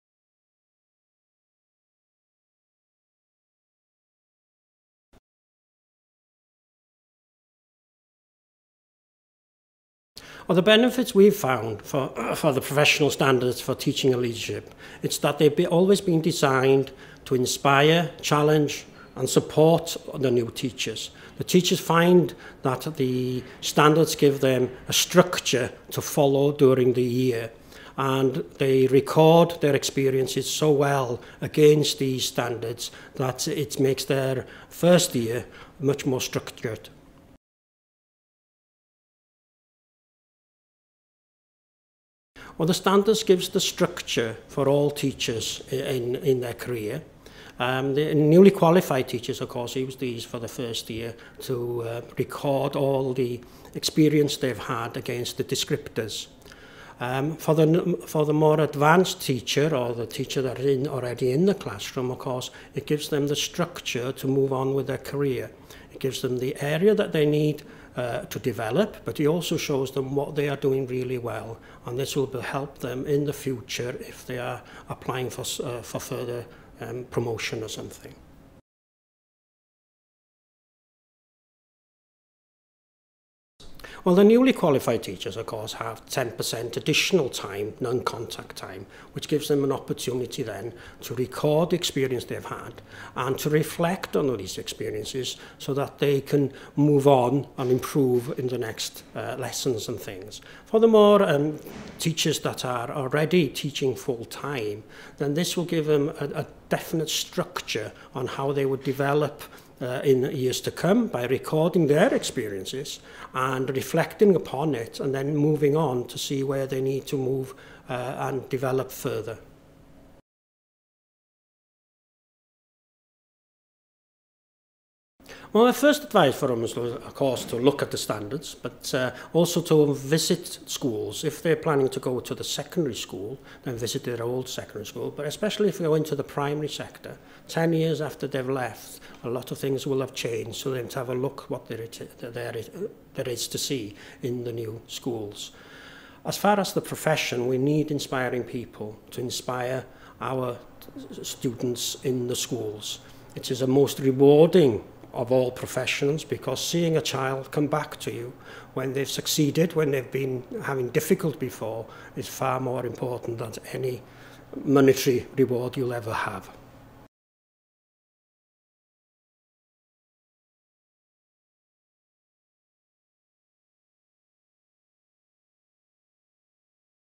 Eifion Roberts - Professional Standards for Teaching and Leadership
Practitioners share experiences of how they use the professional standards to develop their practice.

Mae ymarferwyr yn rhannu eu profiadau o ddefnyddio’r safonau proffesiynol i ddatblygu eu harferion.